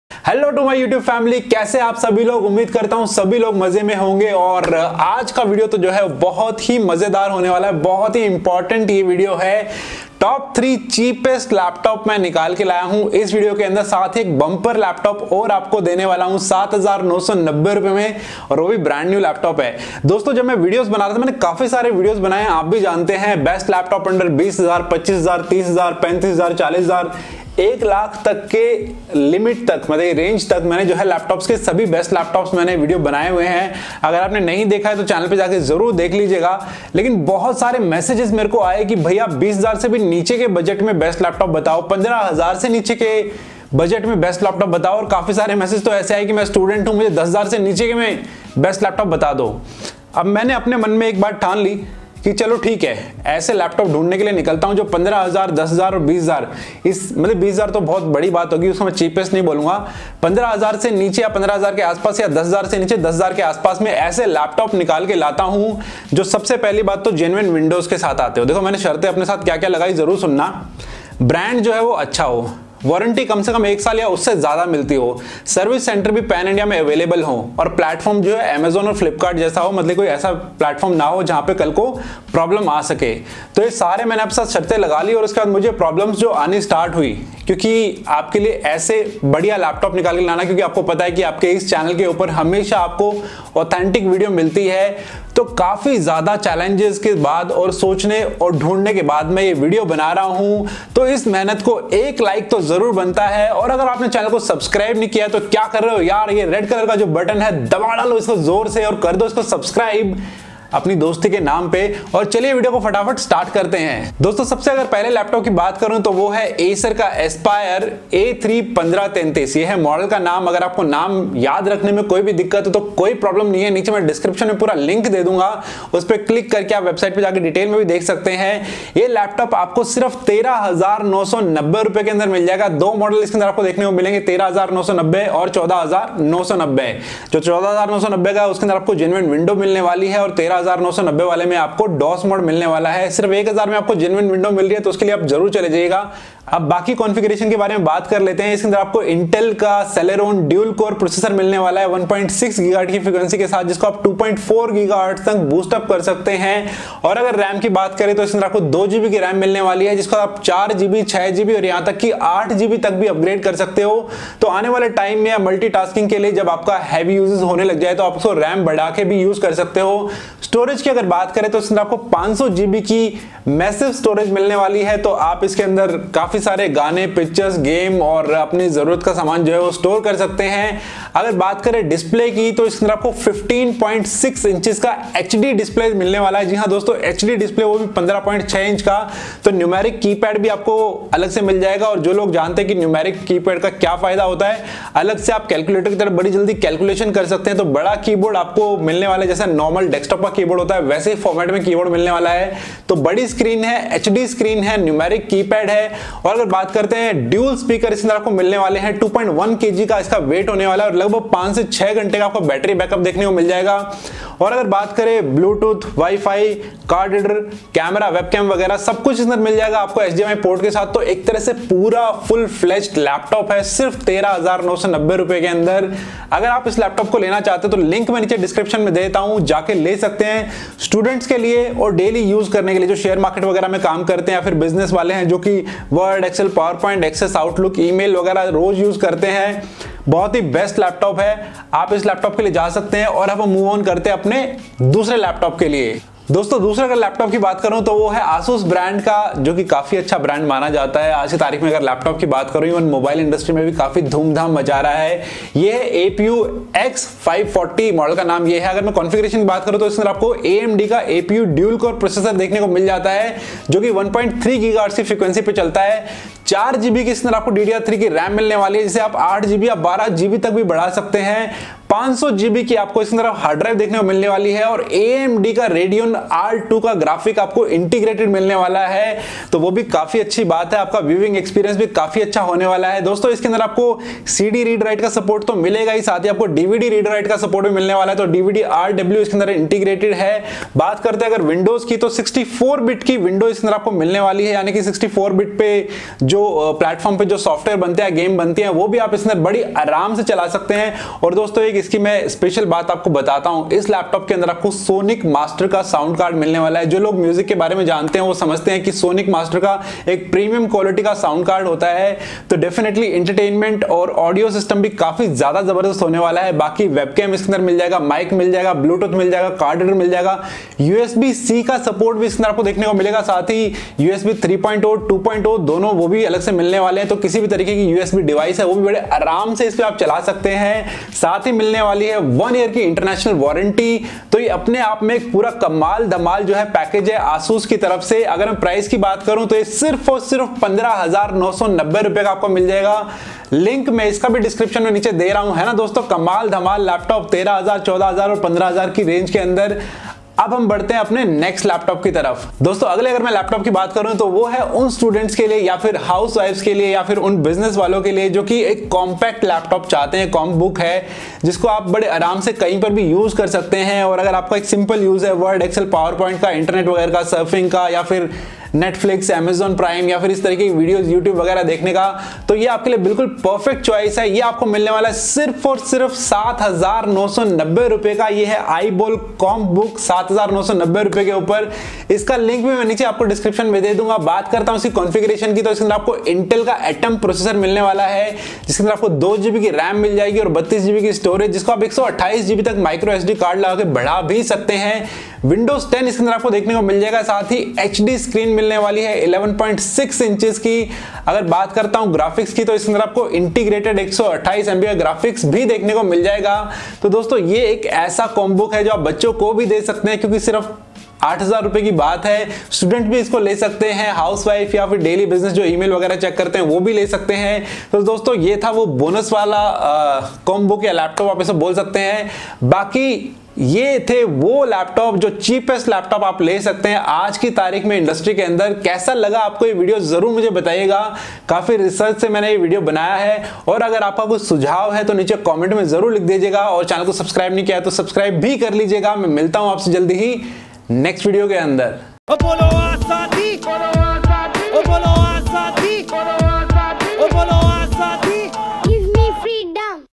Top 3 Cheapest Laptop in India 2019 | Best Laptop Under 15000 and 10000 | Students | Business
1. Acer Laptop

2. Asus Laptop

3. Iball Laptop